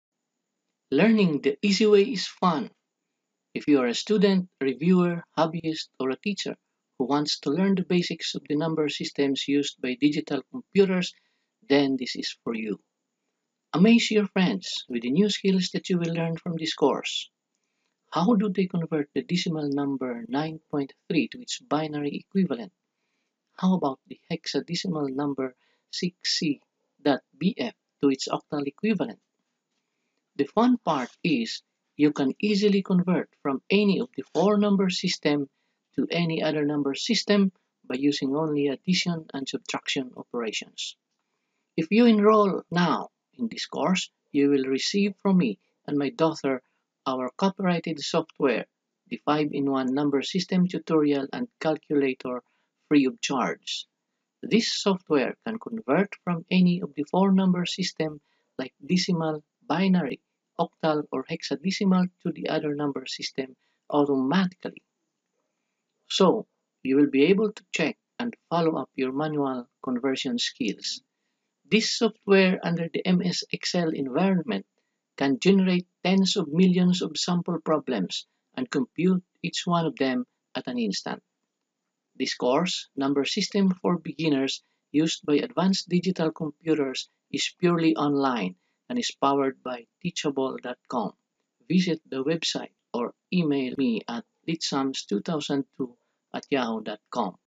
Number System for Beginners: Used by Advanced Digital Computers
This video introduces the course, Number System for Beginners: Used by Advanced Digital Computers and is about the different number systems that are being used in Computer System Design and Programming, and Digital Circuits and Techniques.